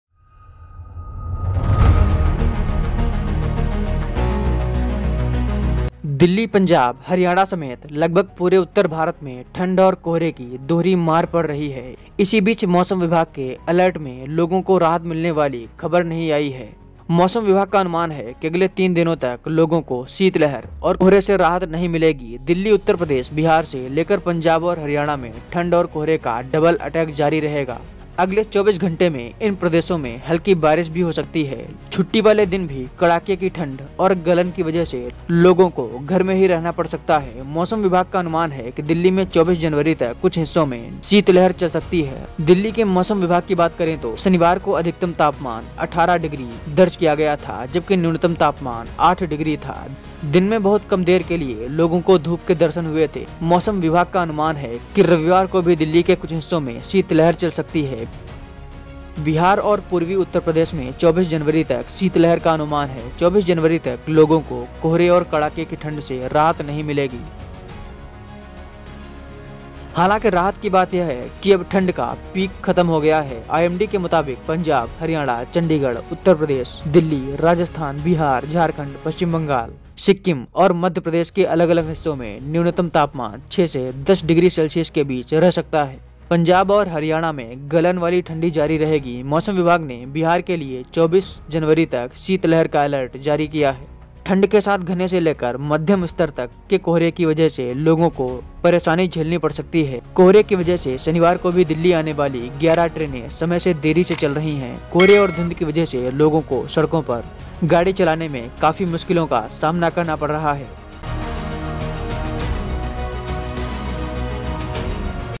Weather Update: अभी मौसम से रहम की उम्मीद न करें मौसम विभाग ने क्या बताया | Hindi News
दिल्ली, पंजाब-हरियाणा समेत लगभग पूरे उत्तर भारत में ठंड और कोहरे की दोहरी मार पड़ रही है. इस बीच आईएमडी के अलर्ट में लोगों को राहत मिलने वाली खबर नहीं आई है. मौसम विभाग का अनुमान है कि अगले तीन दिनों तक लोगों को शीतलहर और कोहरे से राहत नहीं मिलेगी. दिल्ली, उत्तर प्रदेश, बिहार से लेकर पंजाब और हरियाणा में ठंड और कोहरे का डबल अटैक जारी रहेगा. अगले 24 घंटे में इन प्रदेशों में हल्की बारिश भी हो सकती है. छुट्टी वाले दिन भी कड़ाके की ठंड और गलन की वजह से लोगों को घर में भी रहना पड़ सकता है . मौसम विभाग का अनुमान है कि दिल्ली में 24 जनवरी तक कुछ हिस्सों में शीतलहर चल सकती है